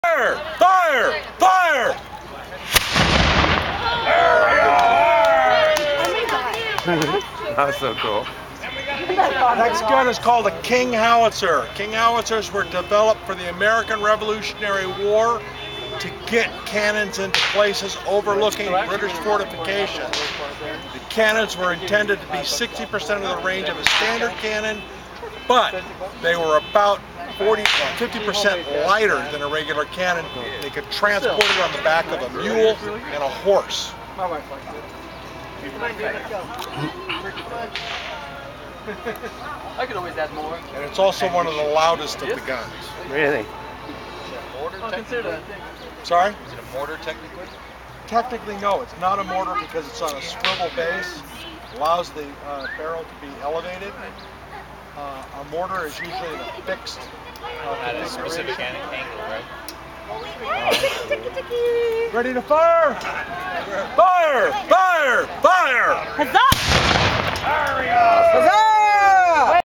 Cannon Fire
Taken at the 2019 Tulare County Renaissance Festival.